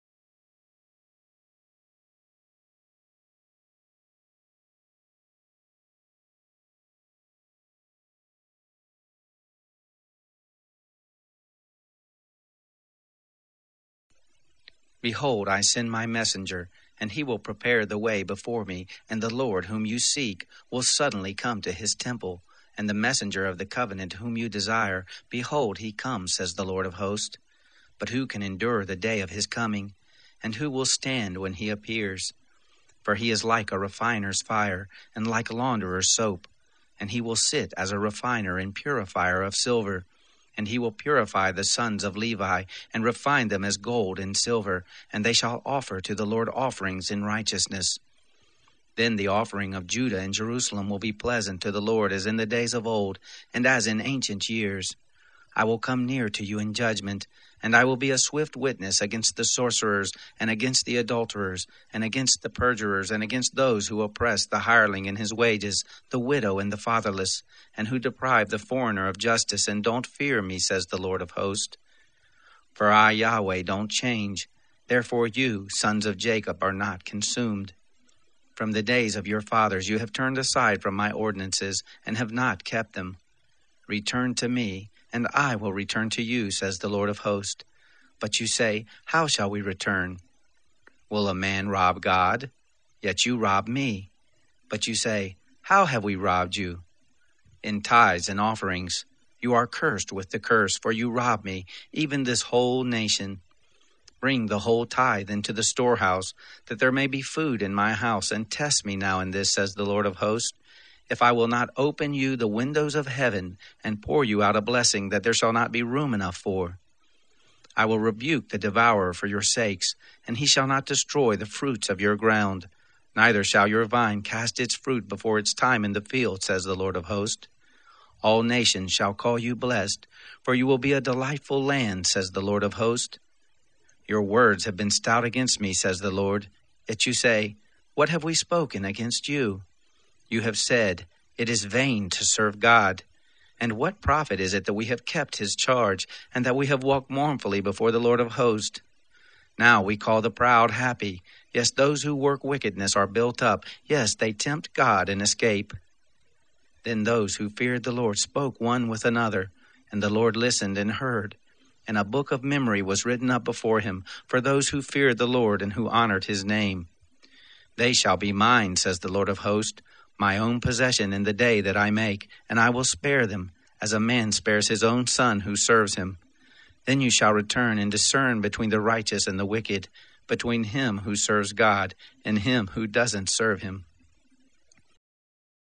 Malachi 3
Chapter 3 of the Book of the Prophet Malachi . Read from the the World English Bible version. This is a public domain version based on the 1901 American Standard Version.